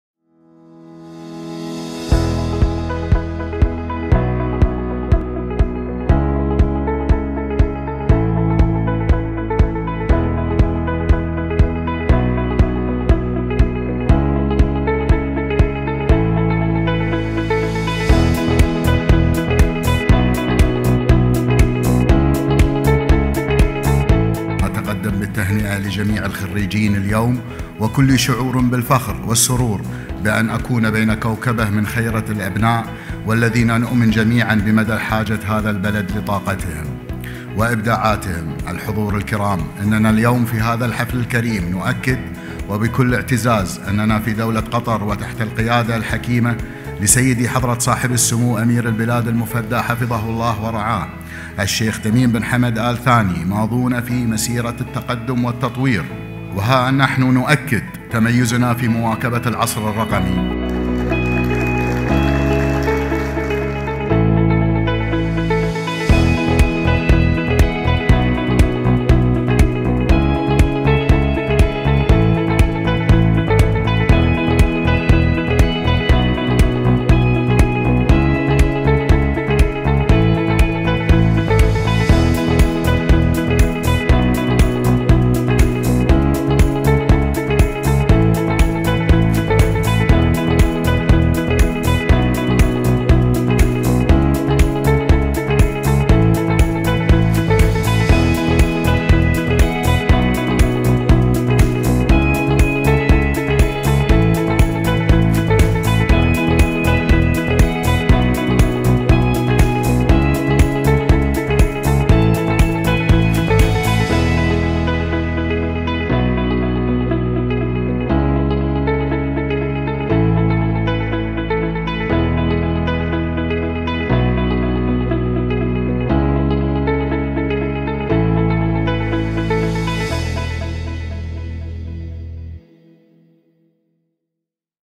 QDGT Highlight Program Annual Event 2018
لقطات من الحفل السنوي لبرنامج حكومة قطر الرّقمية - 2018